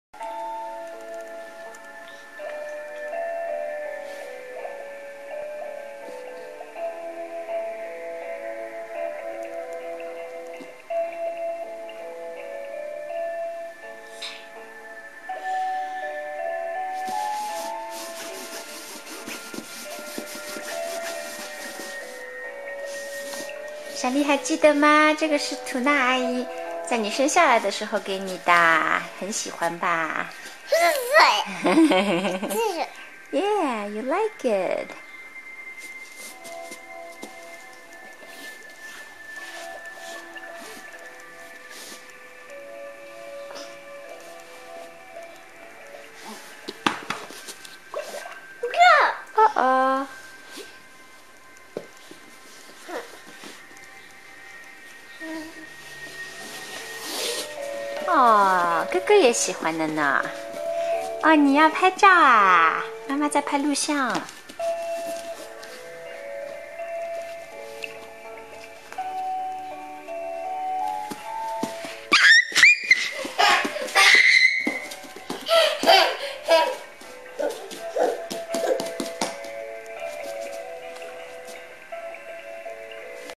Toys from Day 1
For my dear friend Nora and Joe, Lin and Li took a great interest at 16 months of the toys they got from Aunt Nora and Uncle Joe!